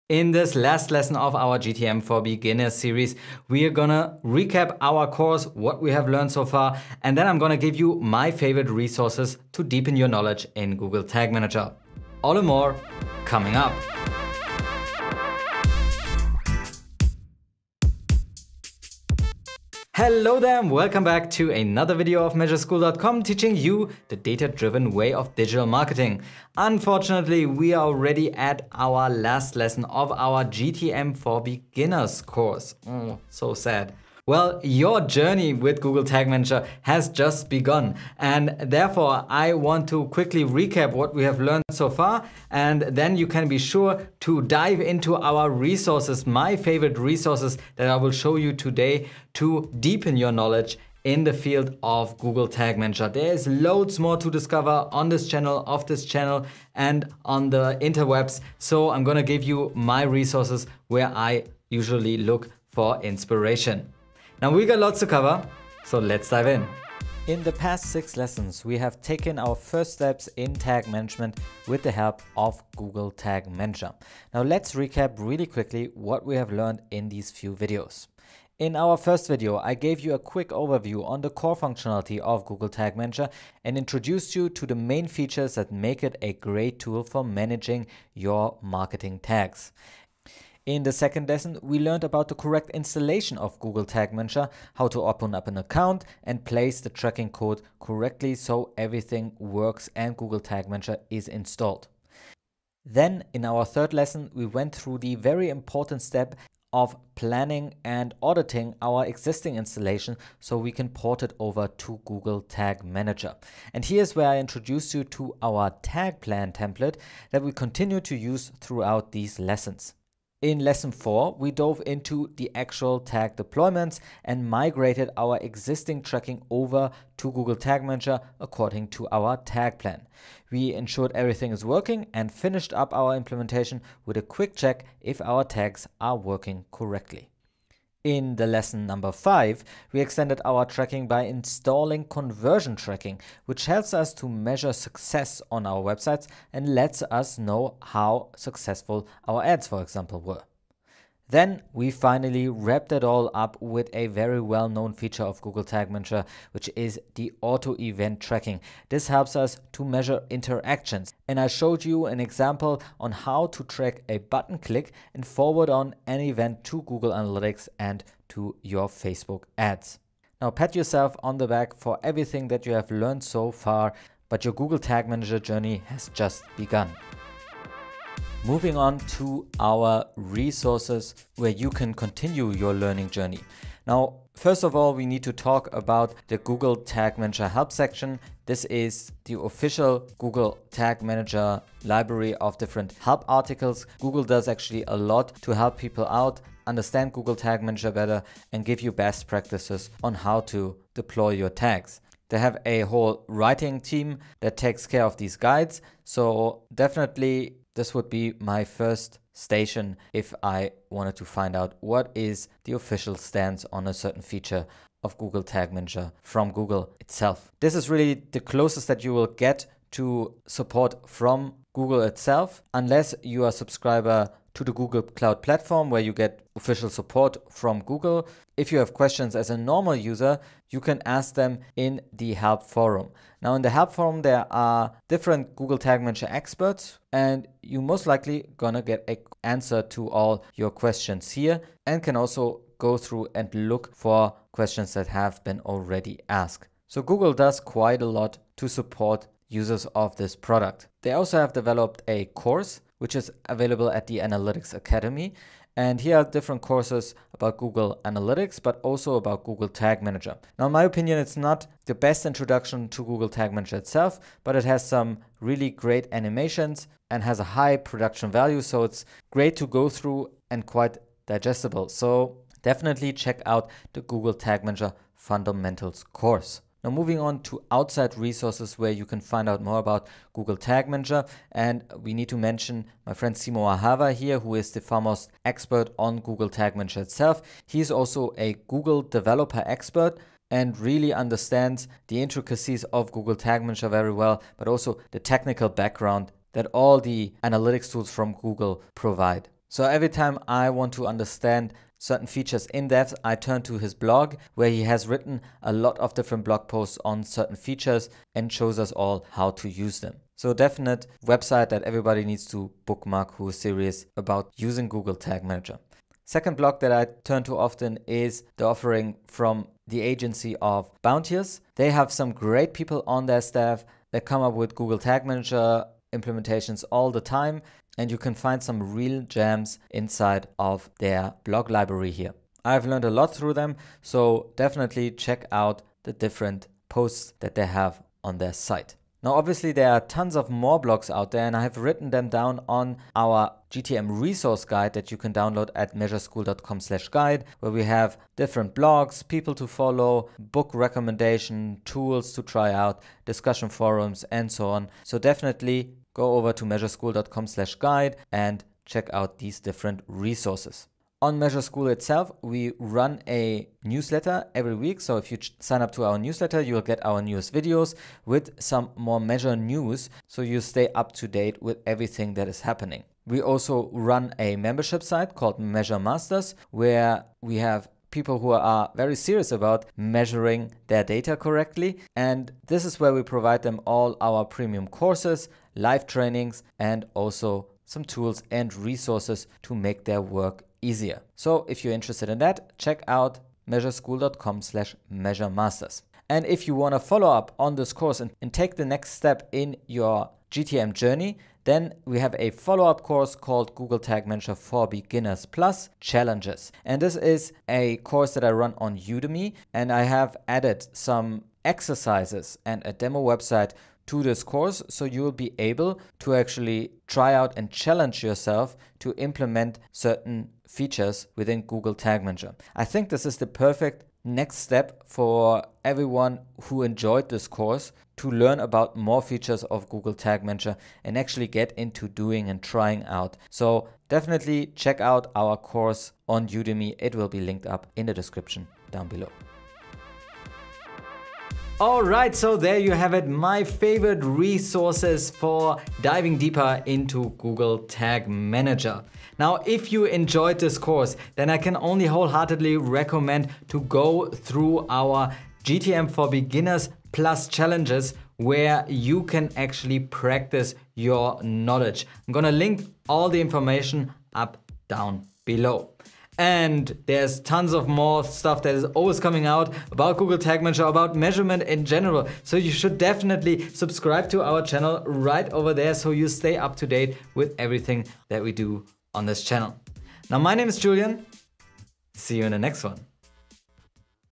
Best Google Tag Manager Blogs and Resources | Lesson 7 (GTM for Beginners)
We have come to the end of our Google Tag Manager for Beginners Series. In today’s video, let’s recap everything that we have discussed and I will also share with you my favorite resources that will guide you throughout your future GTM journey.

OUTLINE:

00:00 - Intro
1:07 - Quick Recap of the GTM for Beginners Course
2:57 - Best GTM Resources
3:04 - Google Tag Manager Help Section/Forum
4:11 - Google Academy
4:42 - Simo Ahava
5:16 - Bounteous
5:40 - MeasureSchool GTM Resource Guide
6:14 - The MeasureMasters Membership
6:38 - Udemy (Now only in MeasureMasters)